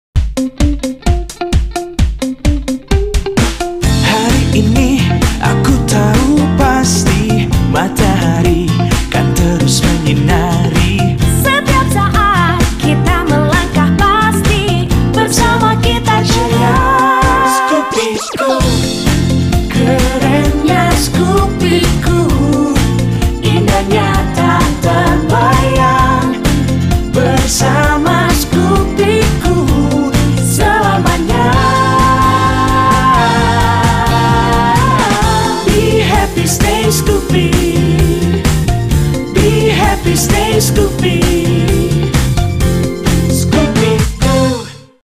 PENEMBAKAN DAN PENGEJARAN PELAKU BEGAL DI JAKARTA VERSI IKLAN HONDA SCOOPY
Pengejaran dan penangkapan pelaku curas di Jakarta (Featuring iklan Honda Scoopy FI)